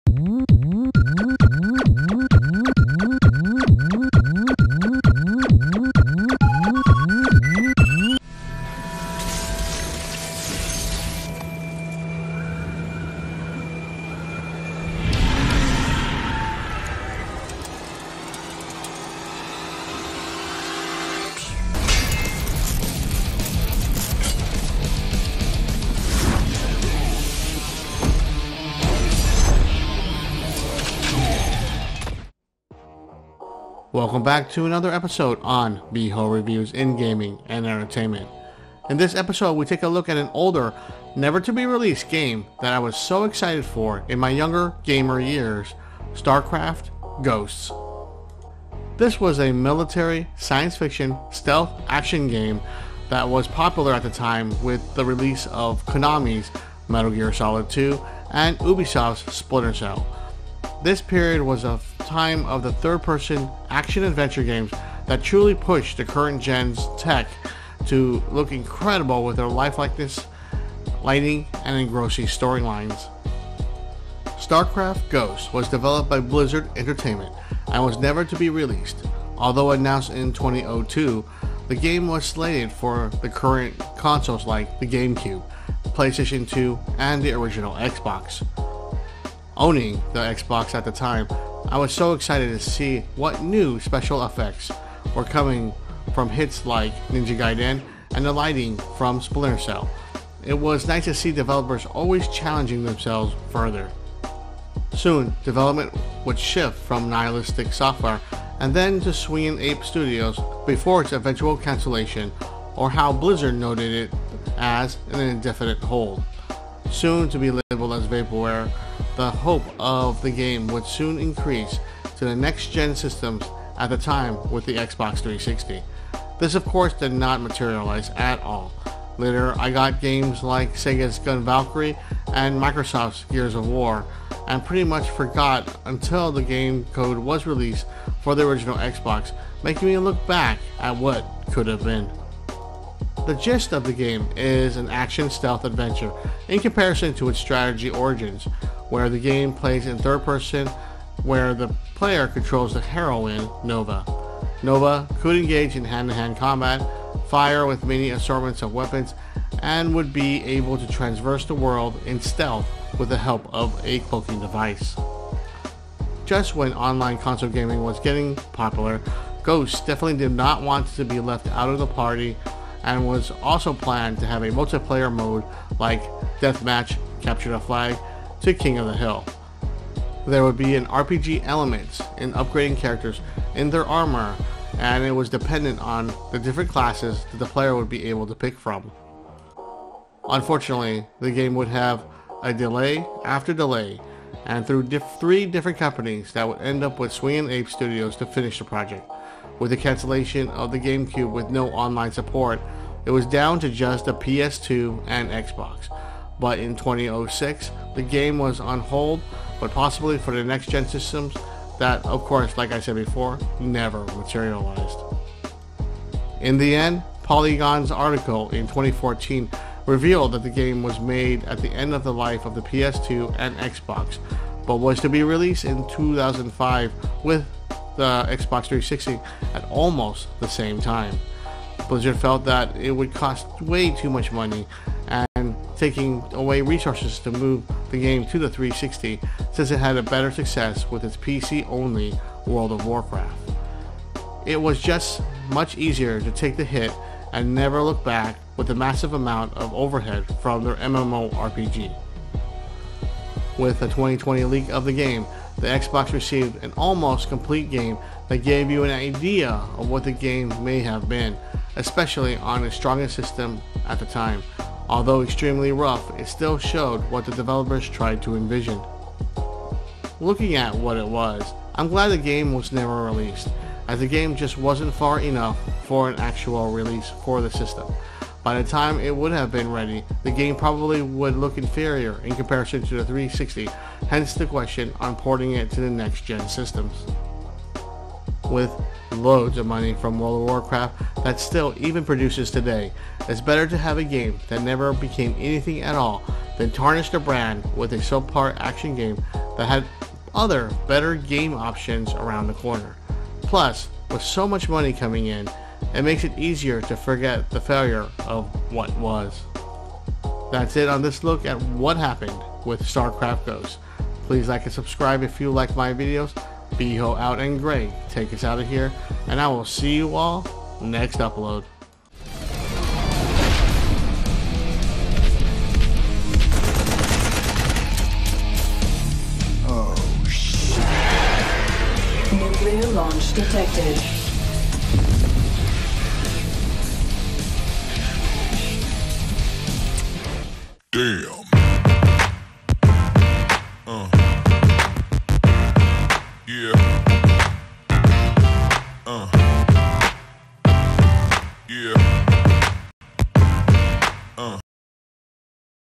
STARCRAFT: GHOST - What Happened
What Happened to old Blizzard Game, StarCraft: Ghost?  StarCraft: Ghost was a military science fiction stealth-action video game developed by Blizzard Entertainment. It was intended to be part of Blizzard's StarCraft series and was announced in September 20, 2002. It was to be developed by Nihilistic Software for the GameCube, Xbox, and PlayStation 2 video game consoles.